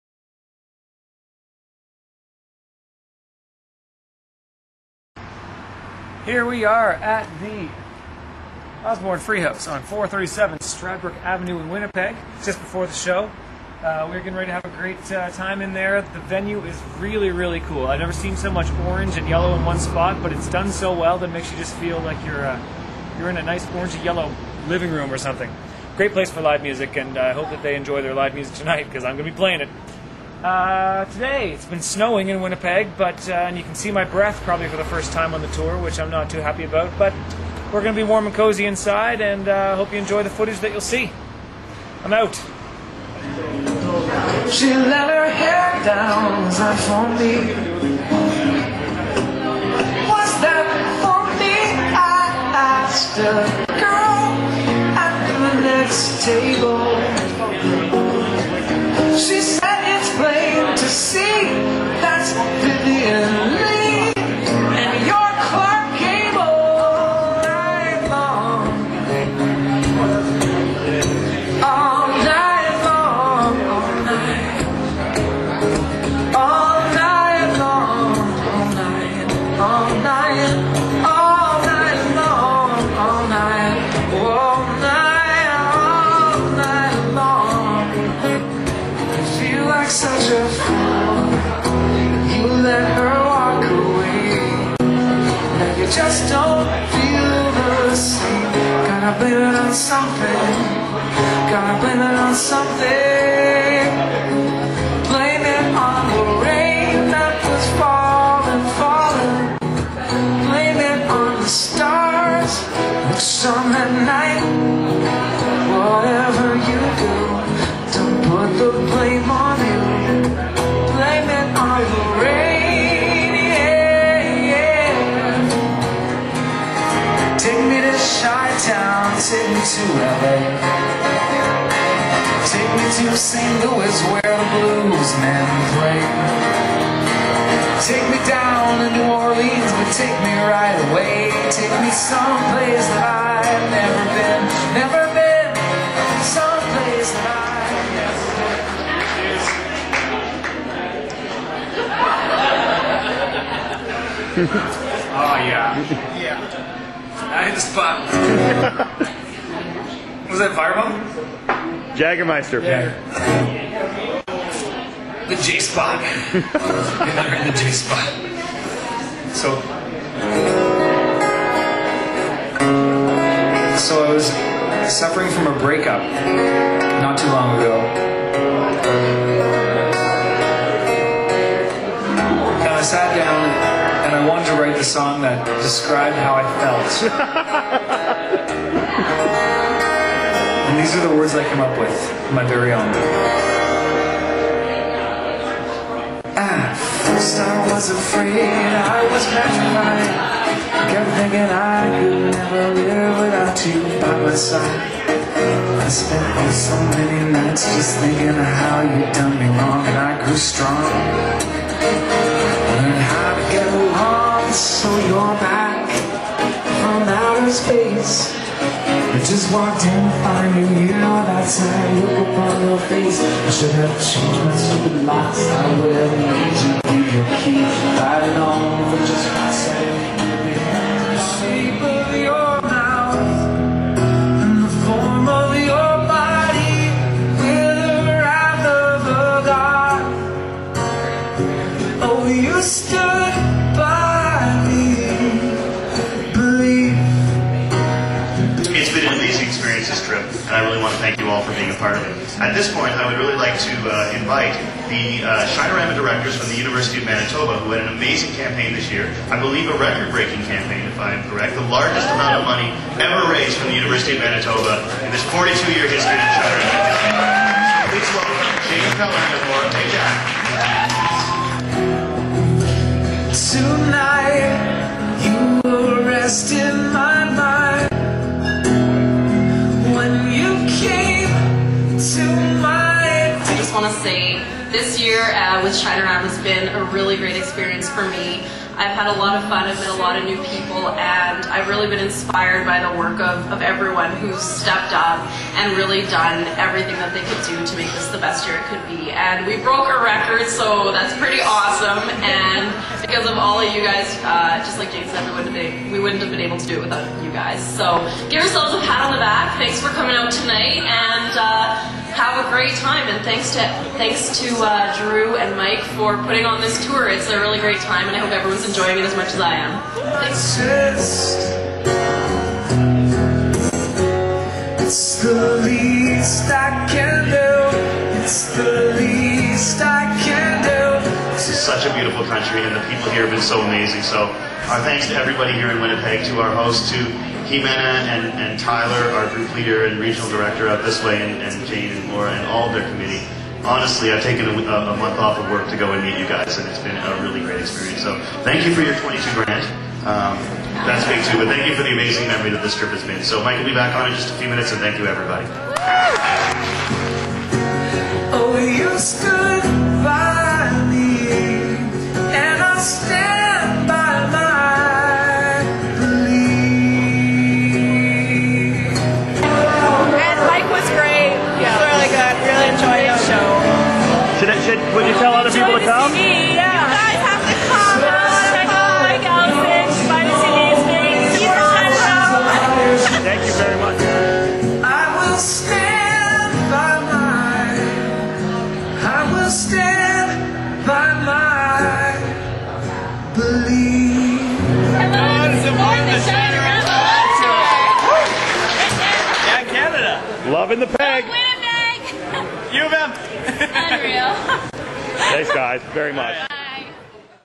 Highlights from the Winnipeg Tour Stop
A look at some of the songs from the Winnipeg show, as well as the record-breaking cheque presentation from Jayne Keller and Laura Payjack, the coordinators of the University of Manitoba's Shinerama campaign.